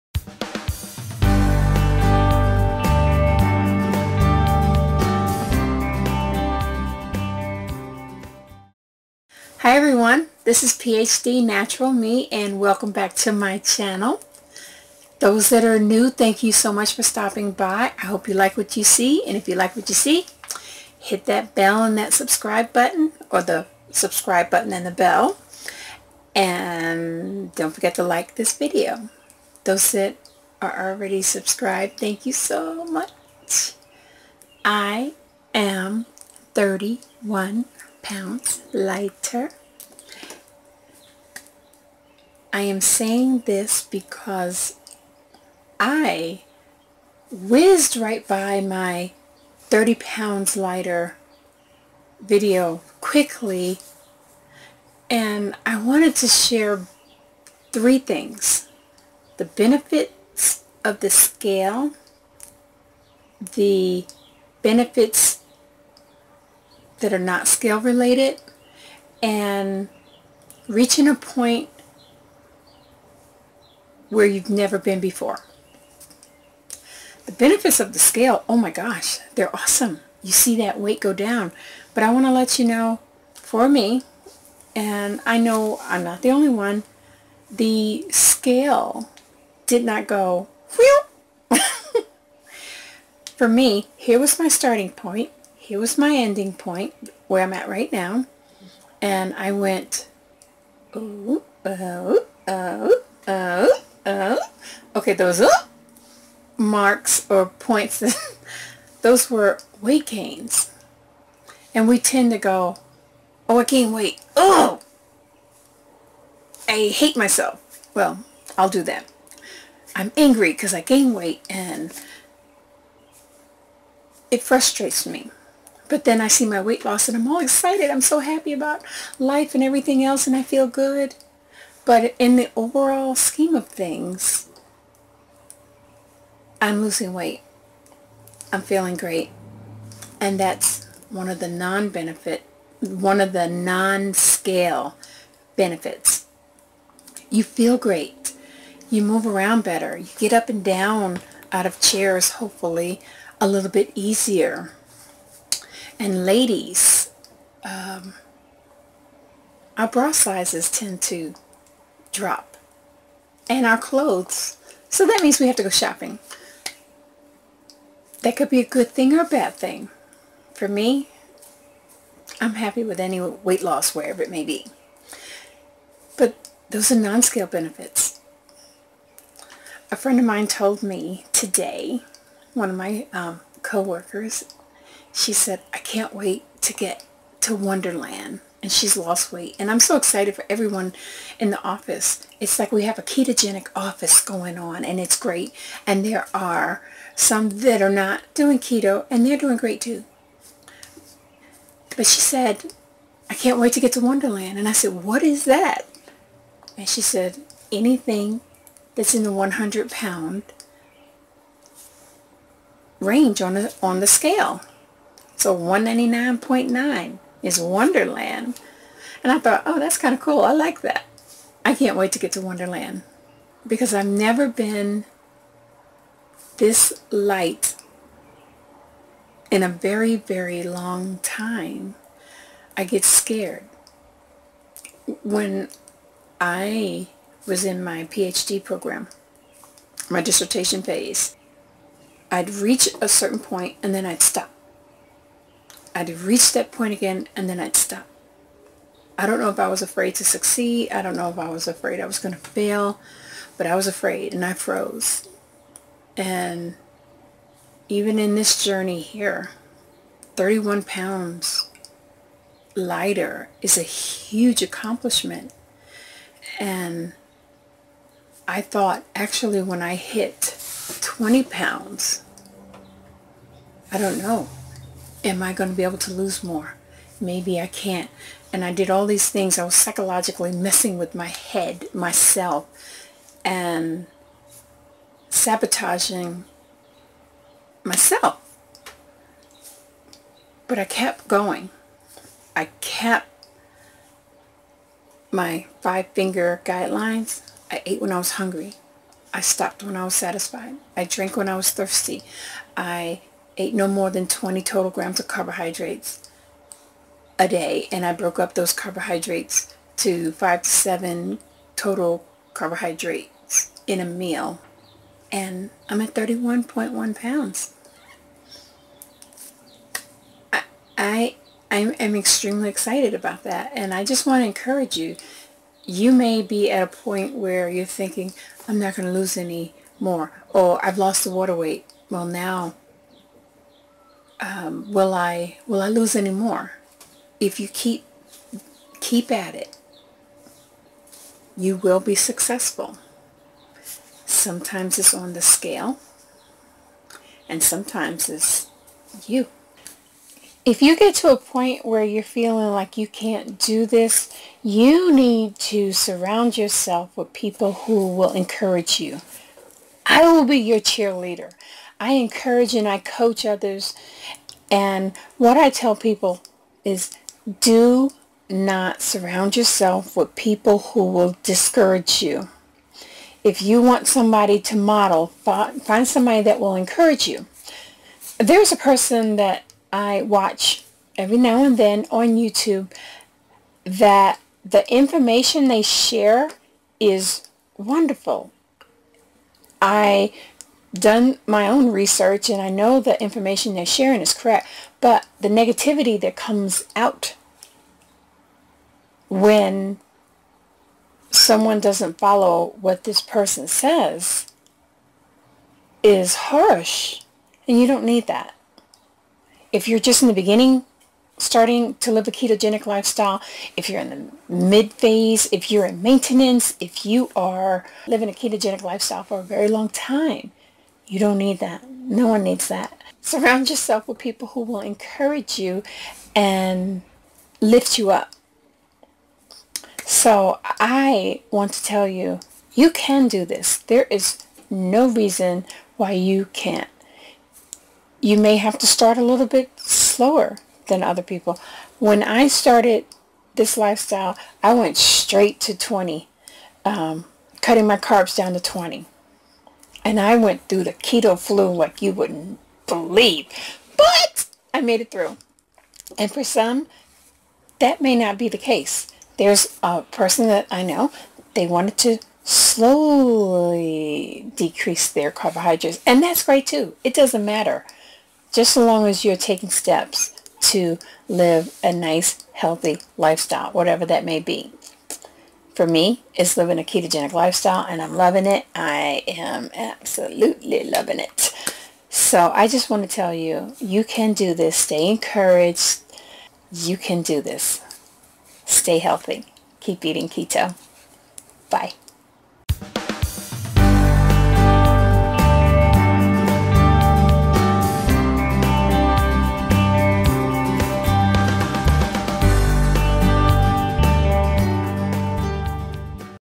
Benefits of Living Ketogenically | The Keto Lifestyle
It’s Dr. Z! PhDNaturalMe. I talk about the benefits of the scale, the benefits that are not scale related, and reaching points in your ketogenic journey that are scary and unexpected.

Stay Healthy and Keep Eating Keto!